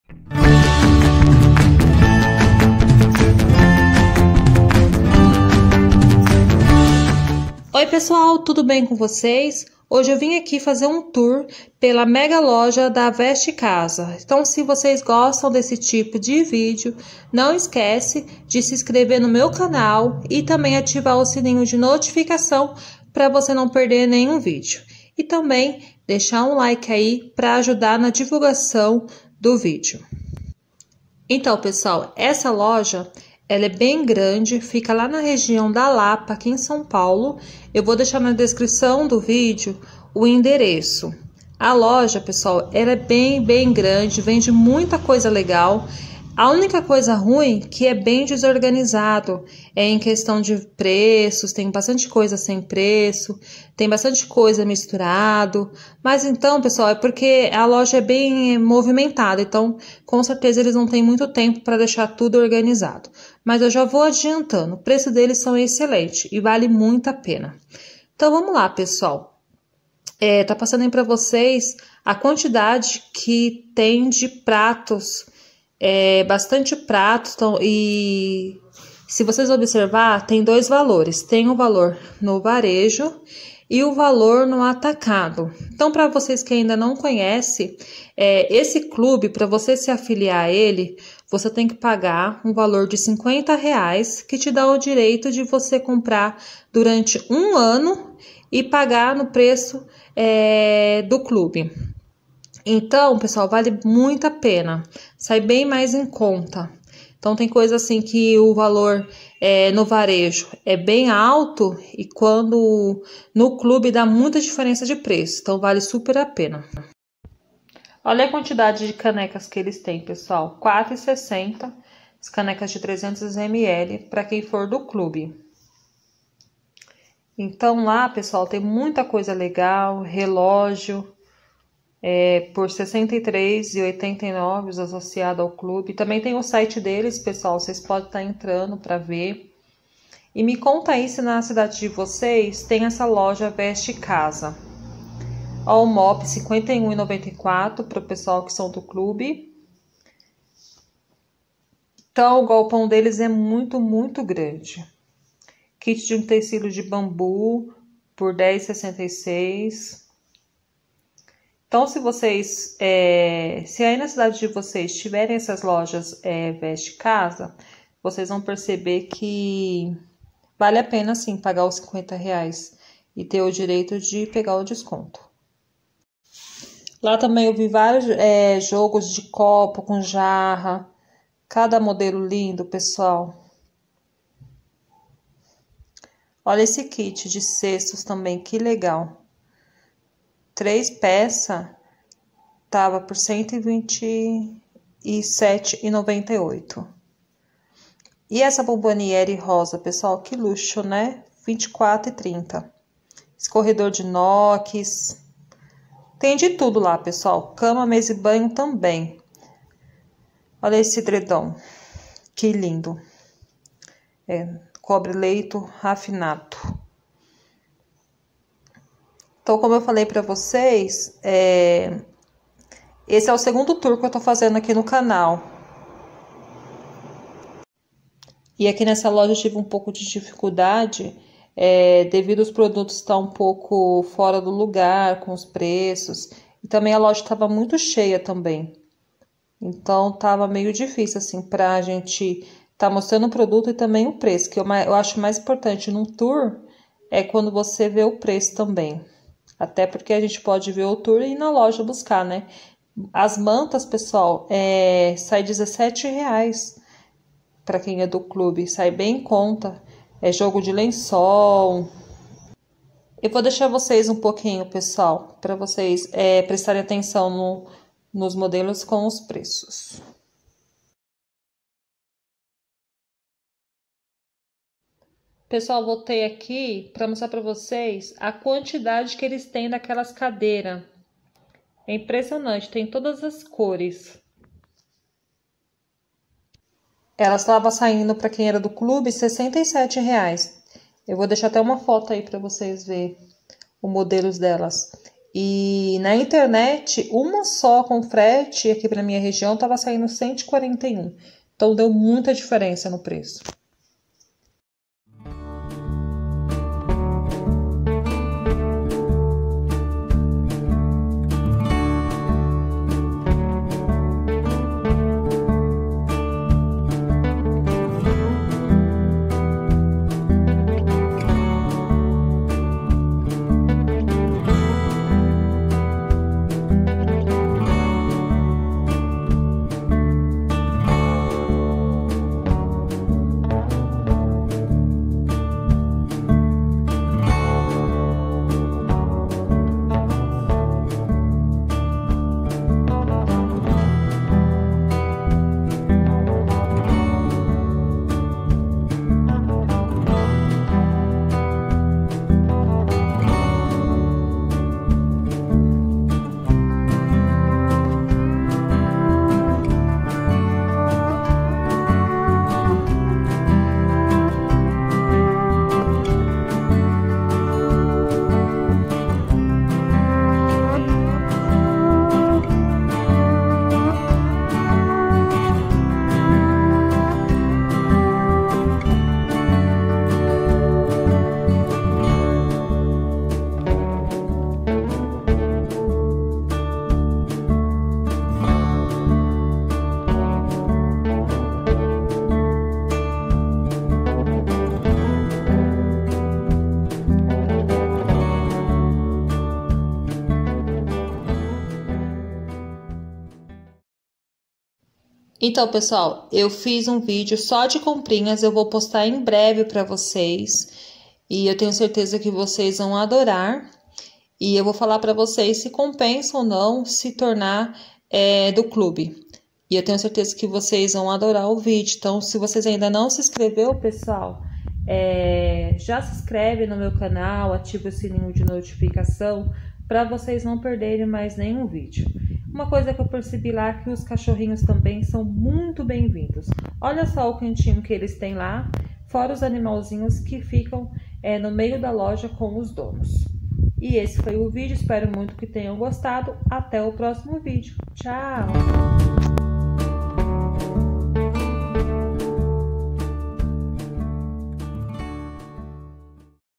TOUR PELA MEGA LOJA VESTCASA DA MARGINAL - Achadinhos para o lar.
Endereço: Av. Ernesto Igel, 429 - Lapa, São Paulo - SP, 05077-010

No vídeo de hoje fiz um tour pela mega vestcasa da Marginal, espero muito que gostem! E me contem aí nos comentários o que vcs acharam dos preços, e o que vcs acharam legal nesse tour. Bjs

Meu Instagram: @cris_gesuato